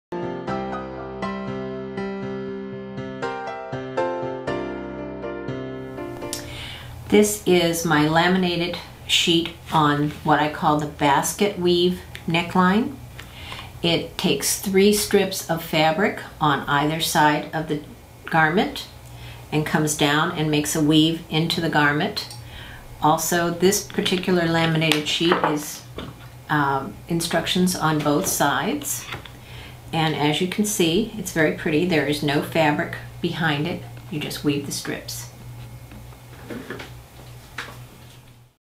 Basket Weave Neckline Laminated Sheet
In this video Emma Seabrooke gives an overview of the Basket Weave Neckline Laminated Sheet.

Emma Seabrooke is a certified Palmer/Pletsch pant and fit instructor. She trained with a French couturier for years and took many fashion design courses in college while obtaining her degree.